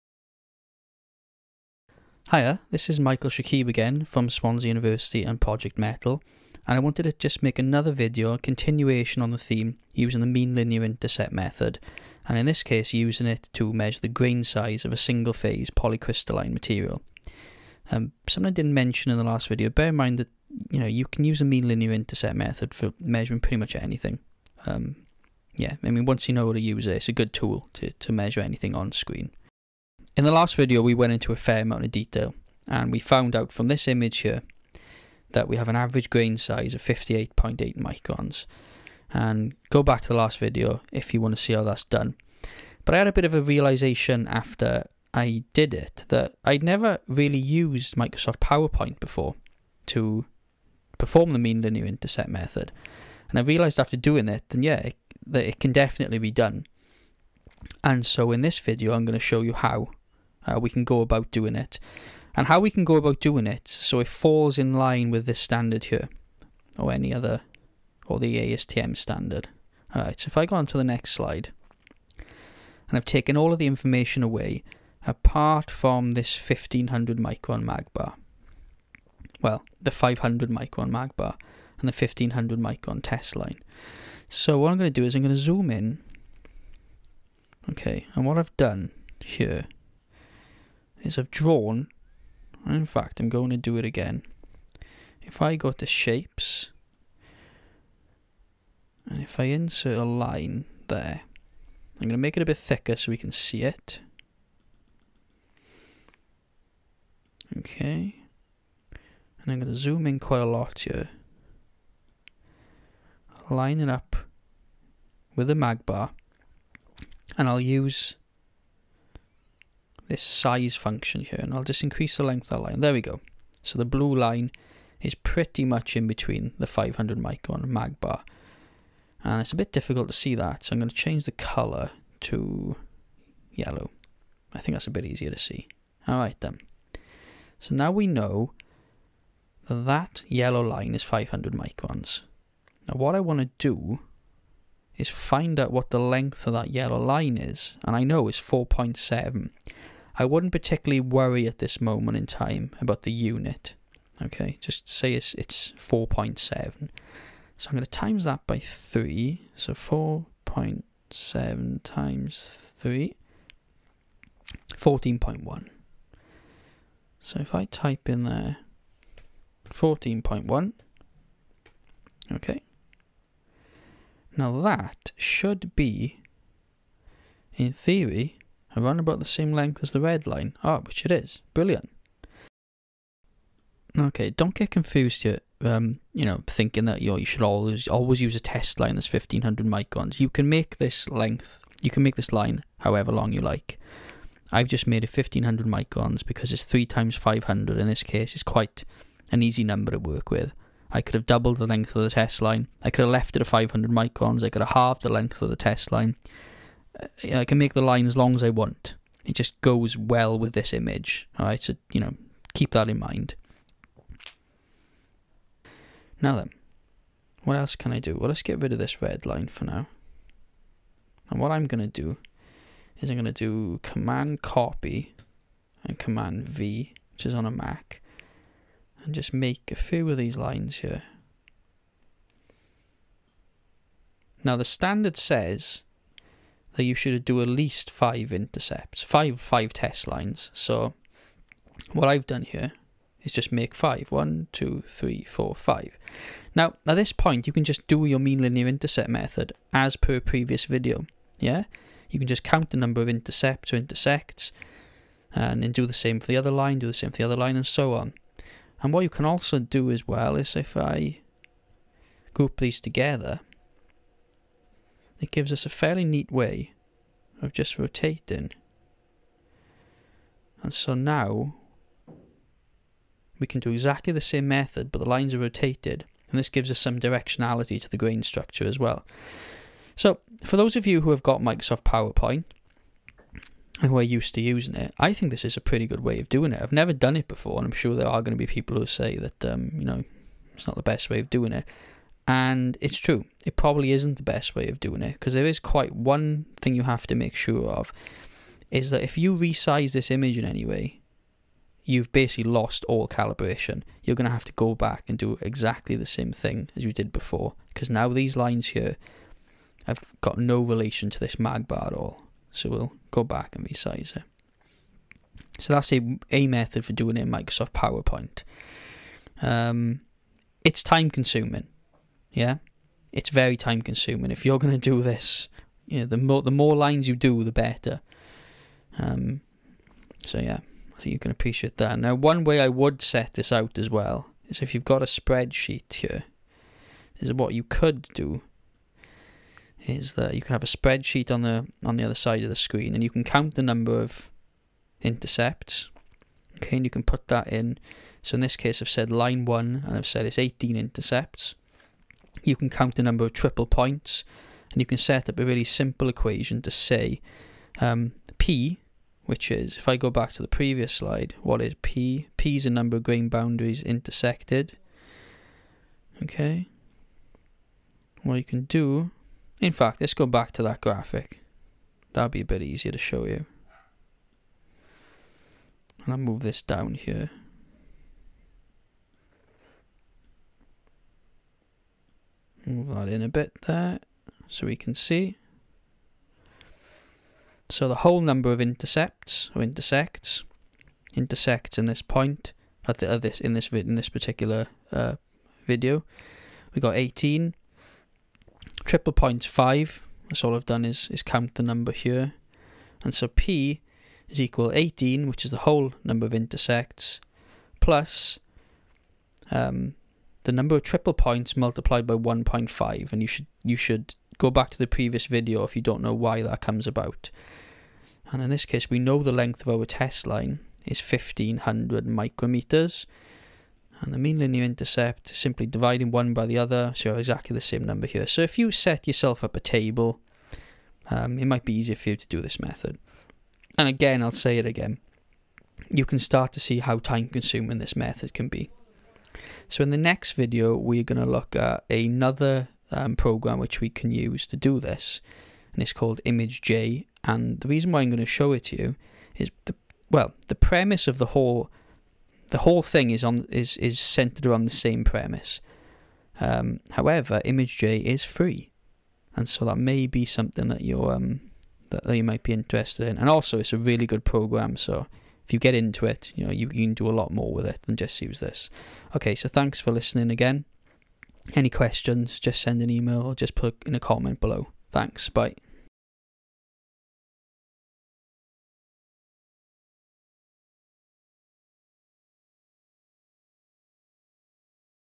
The Mean Linear Intercept Method Using Powerpoint
In this video, Industrial Lecturer Dr Michael Shakib talks about the mean linear intercept method for grain size analysis using Microsoft PowerPoint. It might be seen as an unconventional approach; however, it does still serve as a good way to underpin the theory that we saw in the previous video.